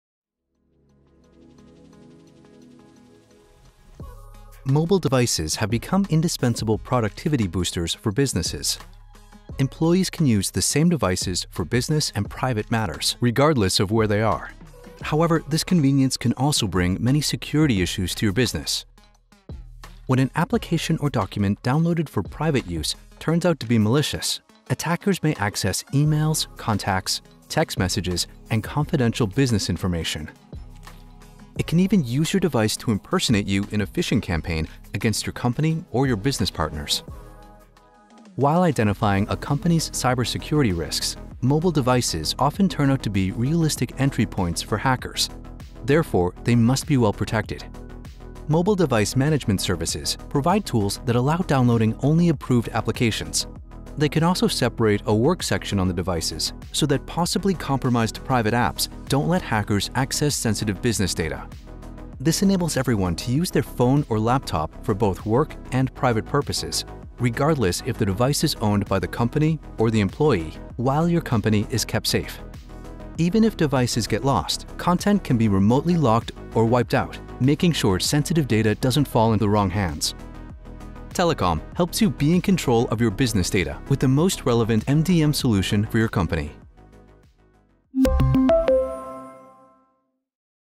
CyberSecured–Pt6–How to protect your business information on mobile devices–Mobile Device Management
These days, we are all reliant on our mobile devices - in fact, you may be reading this on one now! As mobile devices become more frequently used in business, it is important to keep both your professional and personal information secure. If your mobile device has a SecurityBreach, those hackers now have access to your email, contacts and confidential information, and can even impersonate you in PhishingScams from your device.